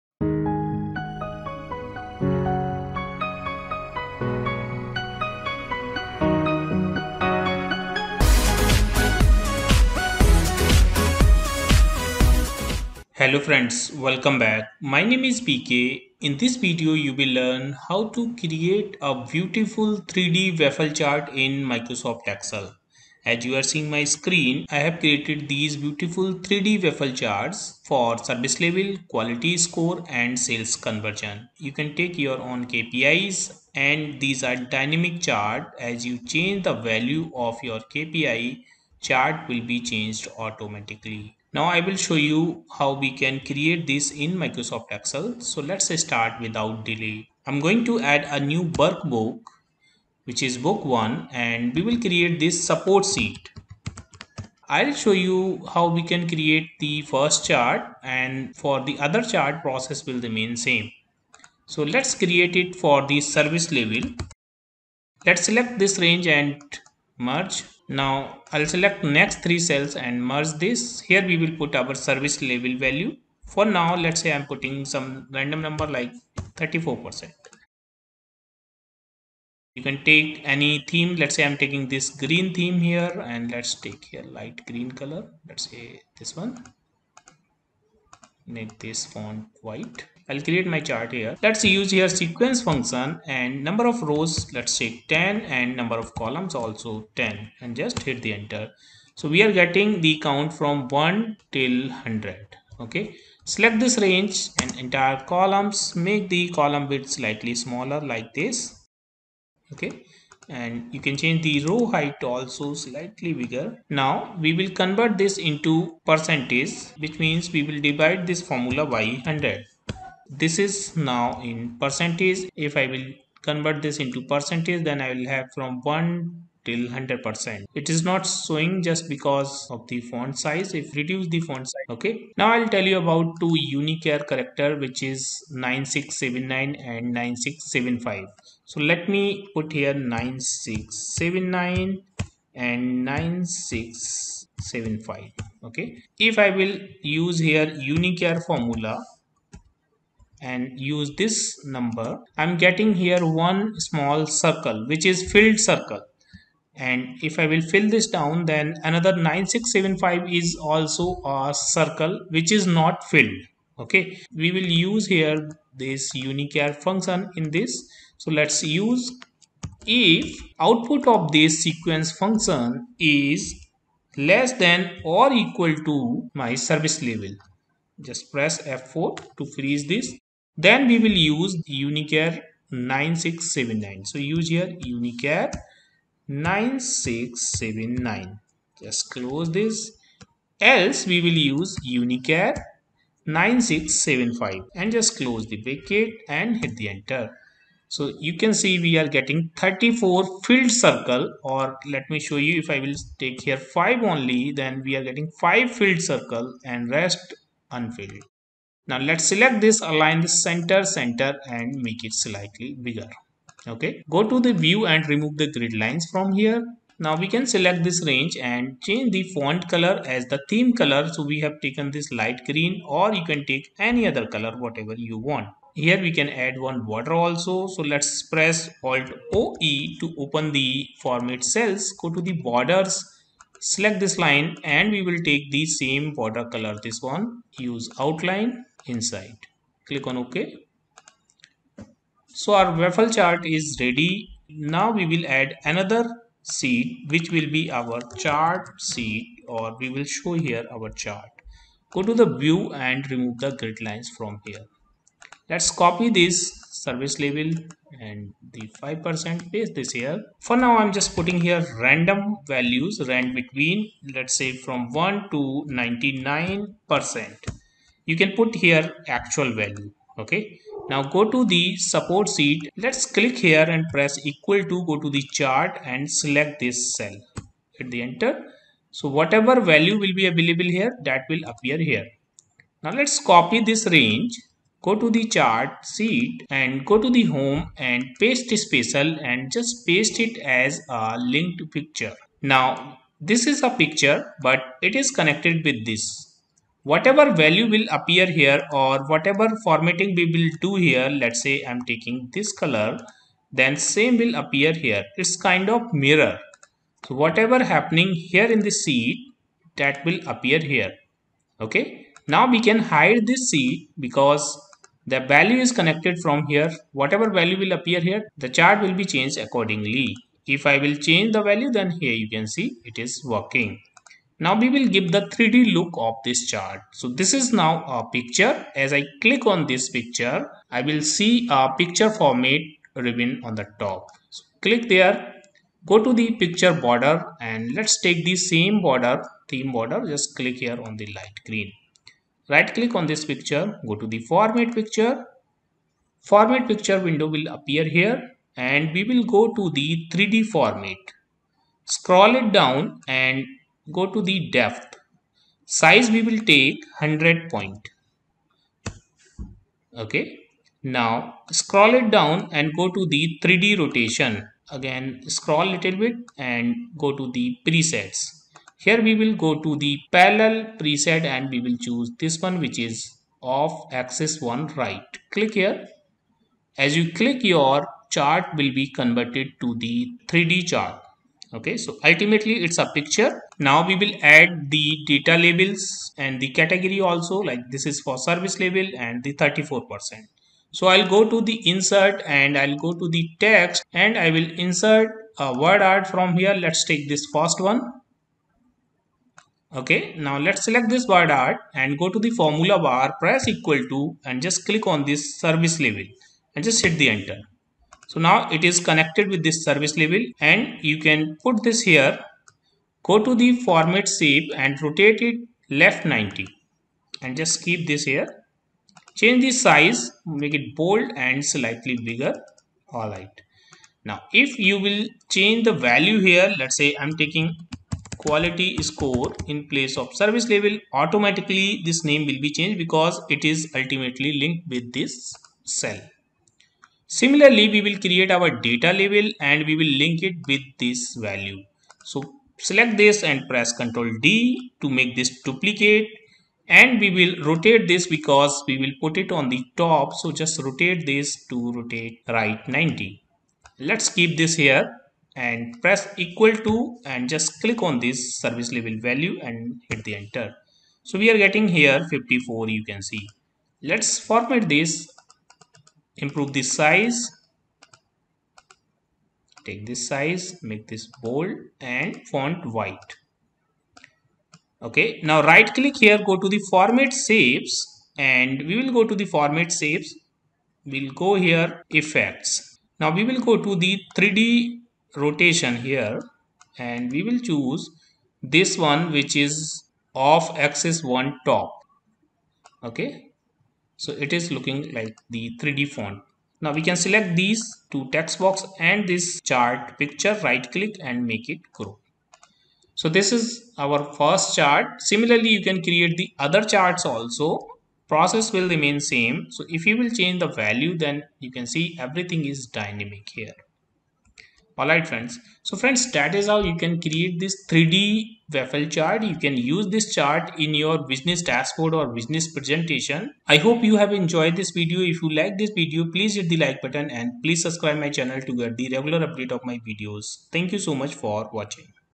Mind-Blowing Excel Hack: 3D Waffle Charts in Just Steps
Hello Friends,

In this video, you will learn how to create 3D Waffle chart in Microsoft Excel.